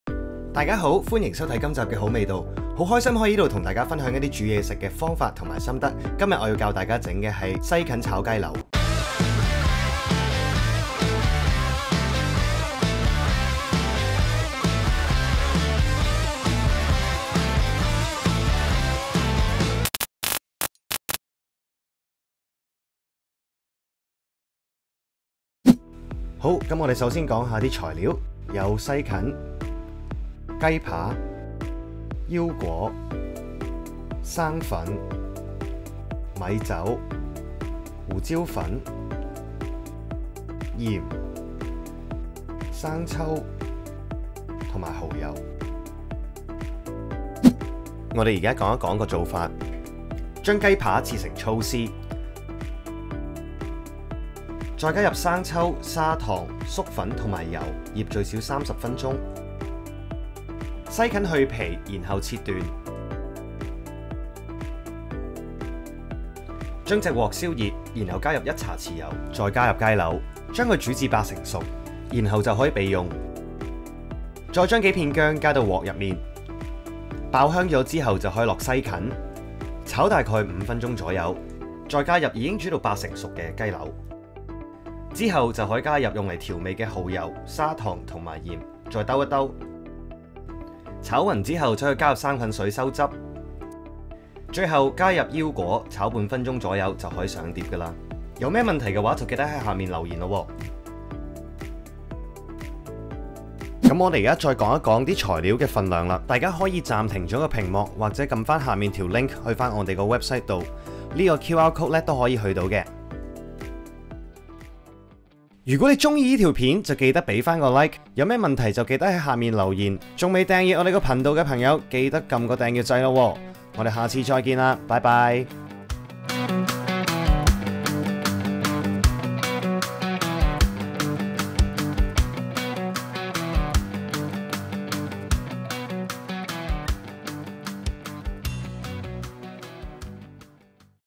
【好味道 S01E16】港式大排檔小炒風味 西芹炒雞柳 食譜及做法
好味道 西芹炒雞柳

材料
300 克 雞肉
 300 克 西芹
 50 克 腰菓
 1 茶匙 砂糖
 1 茶匙 鹽
 2 茶匙 生抽
 1 茶匙 蠔油
 ½ 茶匙 胡椒粉
 1 茶匙 油
 4 片 胡蘿蔔
 4 片 薑

記得Follow埋我哋個Facebook同埋Instagram呀!